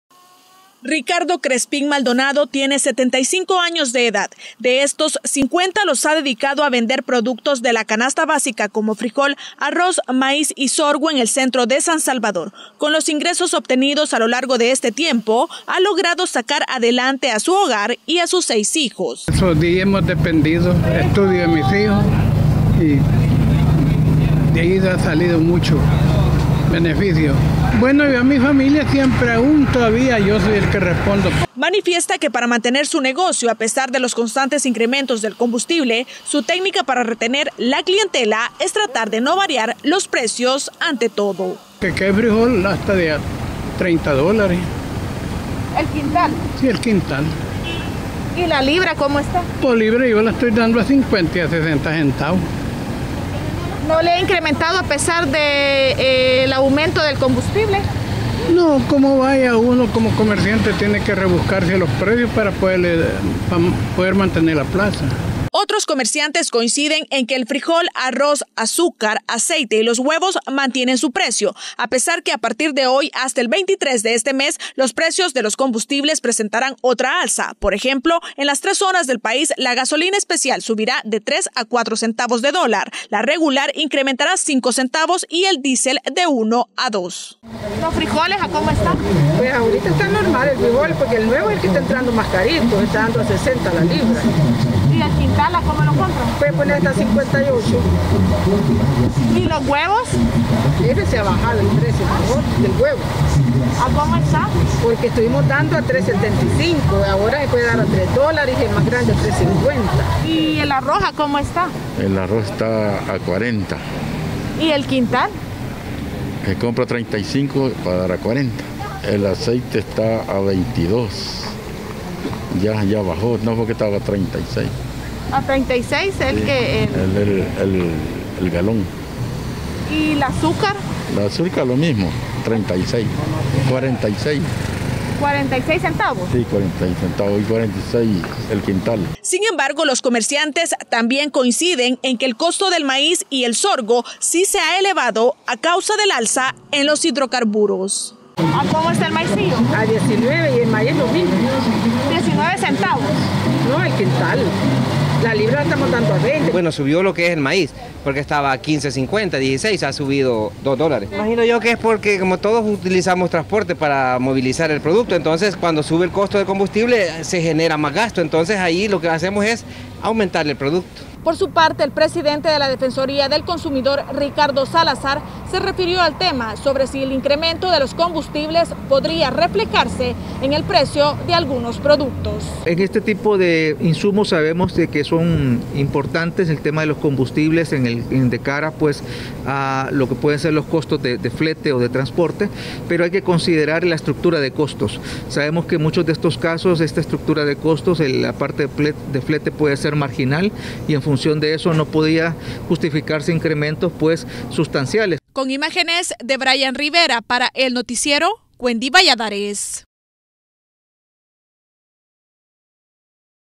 Sondeo de precios en la canasta básica en el centro de San Salvador
A pesar de los constantes incrementos que se han registrado en los combustibles, comerciantes del centro de San Salvador coinciden en que algunos productos de la canasta básica han sufrido aumentos en sus precios, mientras que otros hacen todo lo posible por no elevar los costos en su venta.